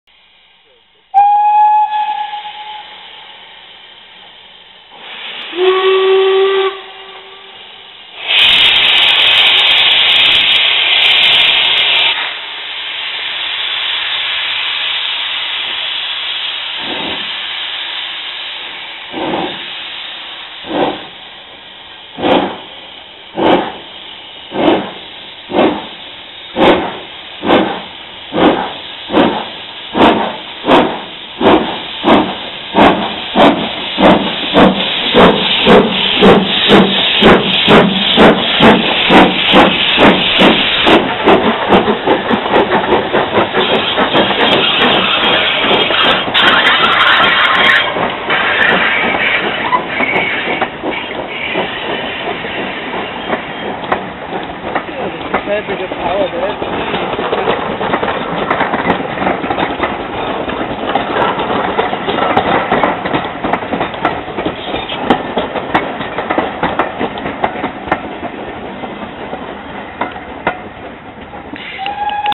BR 73129 & LMS 8P 46233 'Duchess of Sutherland' depart Swanwick Jnc, 14/7/2012
73129 and 6233 double head a train out of Swanwick during 60's weekend